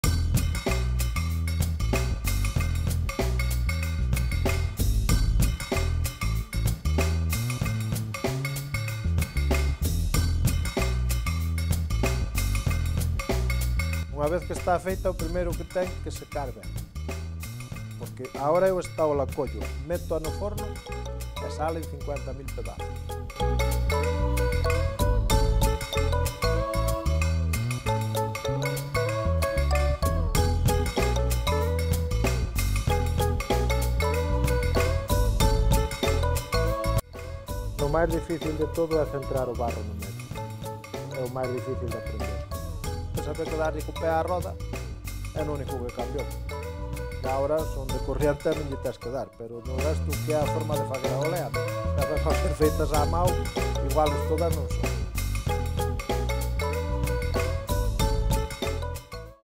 Un artesán de Niñodaguia fai máis de 300 olas para o Domingo Oleiro de Xinzo
Trátase dun barro único ao que Enrique leva dando forma dende hai máis de catro décadas. As súas mans aprenderon o oficio doutros cacharreiros de Niñodaguia.